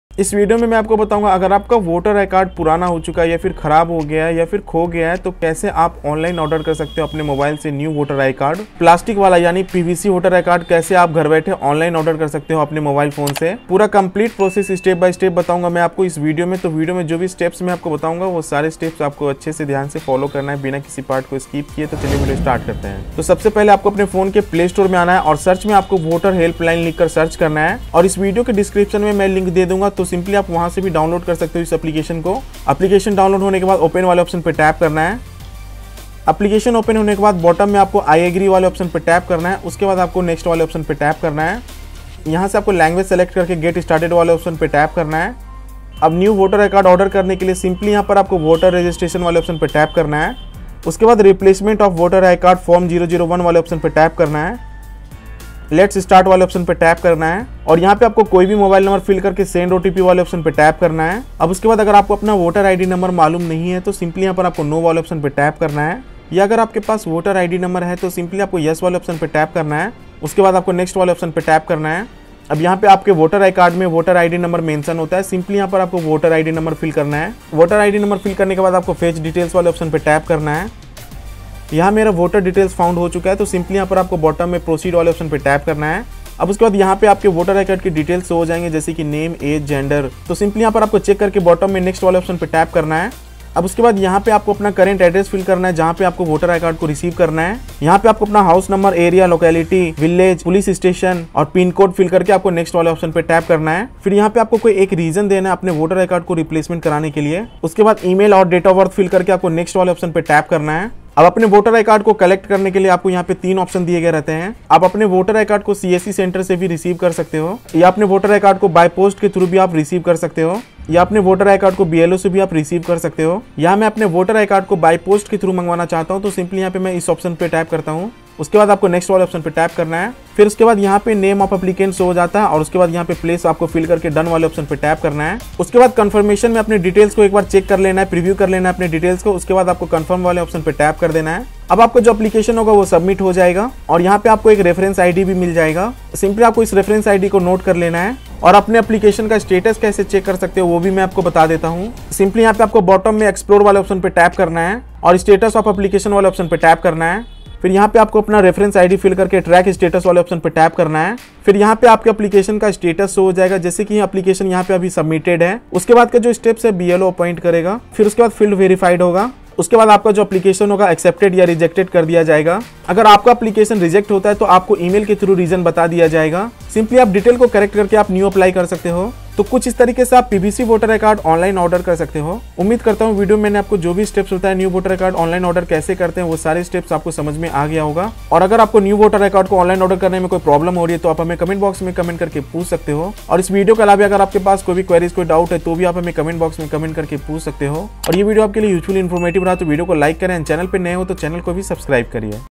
new voter id card apply online | pvc voter id card apply online | 2022
Is video me maine bataya hain aap new voter id card kaise order kar sakte ho online ghar baithe.

About Voter Helpline App

Taking forward its continuous efforts of building an active democratic citizenry in the country, Election Commission of India has undertaken a new initiative by designing a Mobile Application for developing a culture of avid electoral engagement and making informed and ethical ballot decisions among citizens of the country. The app aims to provide a single point of service and information delivery to voters across the country. The app provides following facilities to Indian voters: